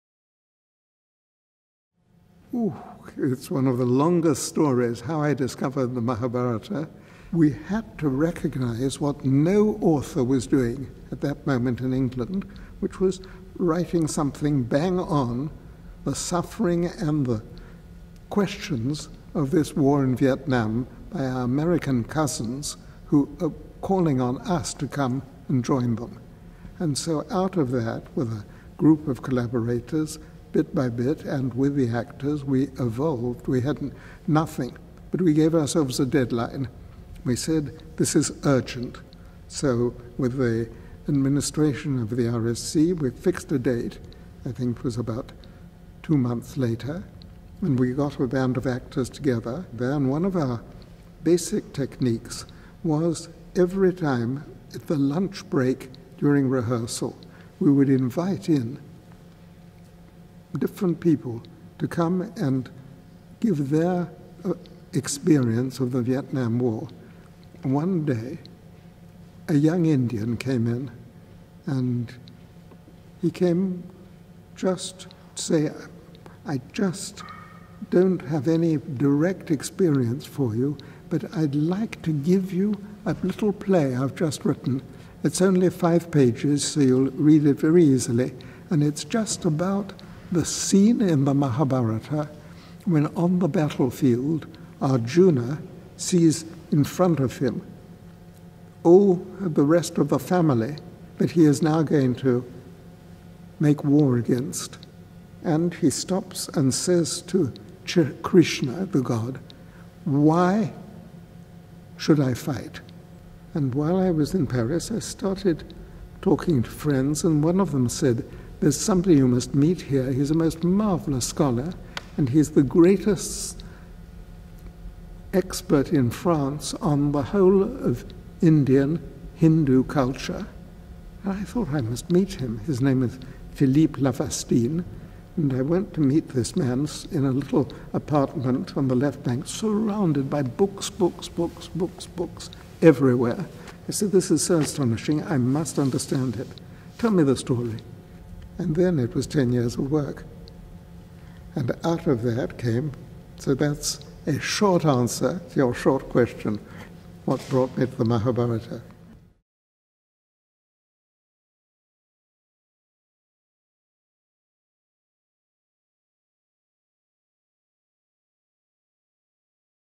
Peter Brook on the making of his 1985 production of The Mahabharata
30 years after it first took the theatre world by storm, Peter Brook discusses the inspiration behind his landmark production of The Mahabharata.
Next February, together with his long-time collaborator Marie-Hélène Estienne, Peter Brook returns to the Young Vic and The Mahabharata with new play Battlefield, which explores one remarkable moment from the original story.

Battlefield plays at the Young Vic from 3 - 27 Feb 2016.